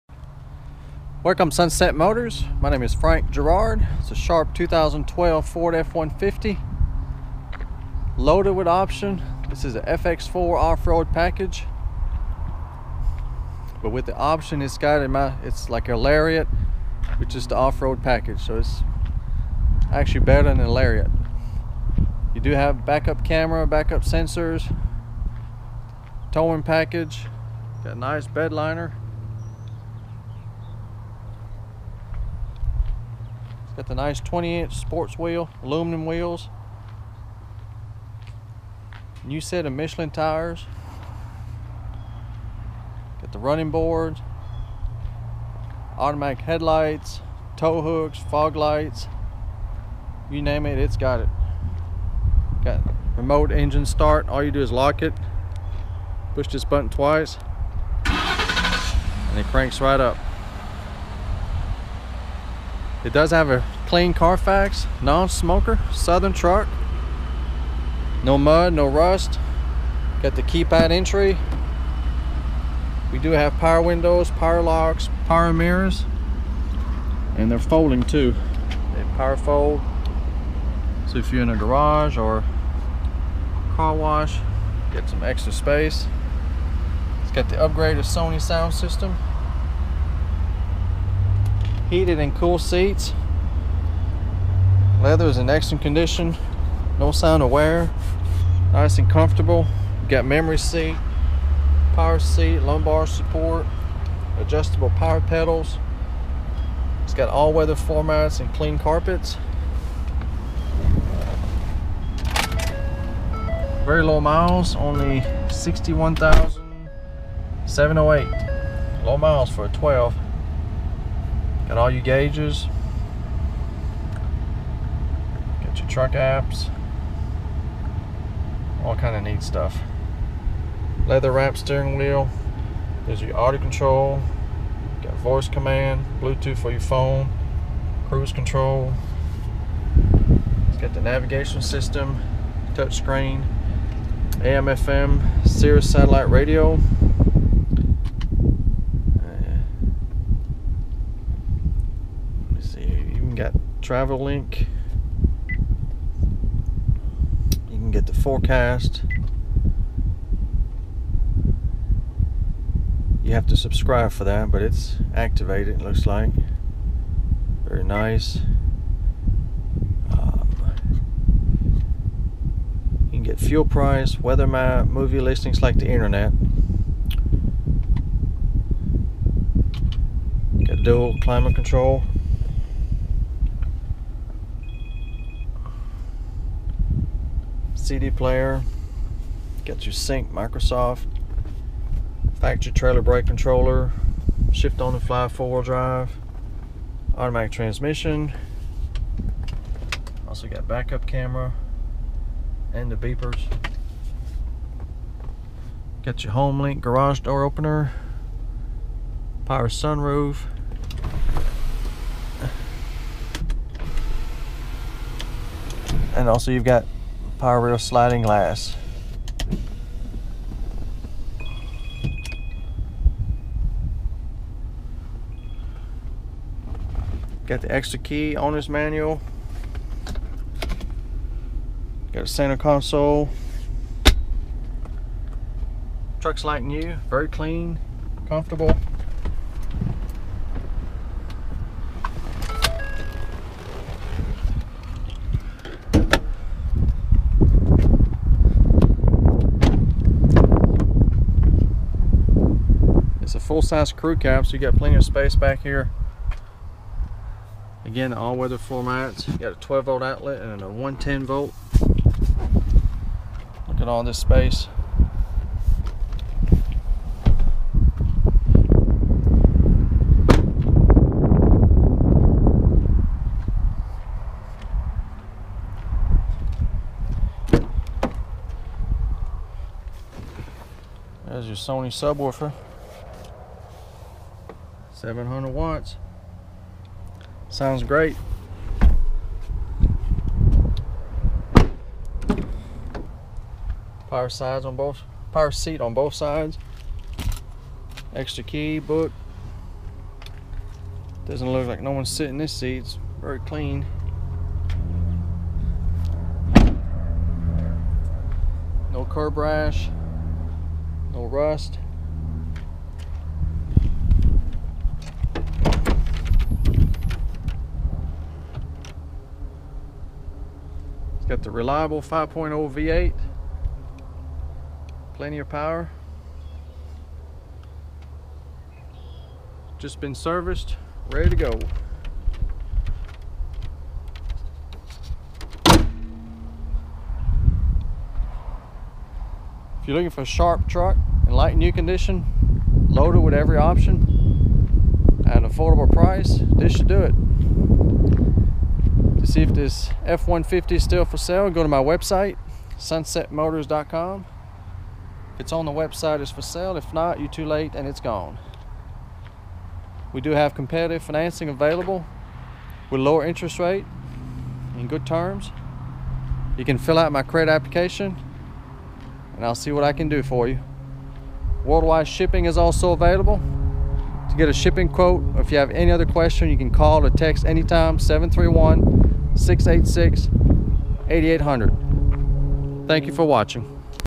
WEST TN 2012 FORD F150 FX4 CREW CAB 4X4 NAV LEATHER SUNROOF FOR SALE LARIAT INFO WWW SUNSETMOTORS CO
FOR MORE PHOTOS INFO AND A WALK AROUND YOUTUBE VIDEO, 1 OWNER CLEAN CARFAX NON SMOKER, SOUTHERN TRUCK, 2012 FORD F150 CREW CAB FX4 OFF ROAD LUXURY PACKAGE, 5.0L V8 FFV ENGINE, 6 SPEED AUTOMATIC TRANSMISSION WITH MANUAL SELECT SHIFT, SHIFT ON THE FLY 4X4, HEATED AND COOLED BLACK LEATHER BUCKET SEATS, DUAL POWER SEATS WITH LUMBAR AND MEMORY, AM/FM/XM/CD/USB/AUX SONY SOUND SYSTEM, TOUCH SCREEN SONY NAVIGATION SYSTEM WITH WEATHER, FUEL PRICES, & MORE, DUAL CLIMATE CONTROL, BACK UP CAMERA AND SENSORS, STEERING WHEEL MOUNTED AUDIO CONTROLS, BLUETOOTH, TRIP COMPUTER WITH TRUCK APPS, CRUISE CONTROL, ADJUSTABLE POWER PEDALS, AUTOMATIC HEADLAMPS, FOG LIGHTS, UNIVERSAL GARAGE DOOR OPENER, POWER MOON ROOF, AUTOMATIC DIMMING REAR VIEW MIRROR, POWER SLIDING REAR GLASS, 110 VOLT OUTLET, REMOTE ENGINE START, KEYPAD ENTRY, POWER FOLDING MIRRORS, TRAILER BRAKE CONTROLLER, TOWING PACKAGE, FX RUNNING BOARDS, 36 GALLON FUEL TANK, BED LINER, 3.73 ELECTRONIC LOCK REAR AXLE, 20 INCH FX ALUMINUM WHEELS WITH NEW MICHELIN TIRES, EXTRA KEY, OWNER'S MANUAL, JUST SERVICED ONLY 57K MILES, ALL WEATHER FLOOR MATS, REMAINDER OF FACTORY WARRANTY 08/12/2017 OR 60,000 MILES DRIVETRAIN, PRICE TO SELL NEW TRUCK LIKE THIS IS OVER $50K !!, COMPETITIVE FINANCING, EXTENDED WARRANTY AND WORLD WIDE SHIPPING IS AVAILABLE, CALL ANYTIME TOLL FREE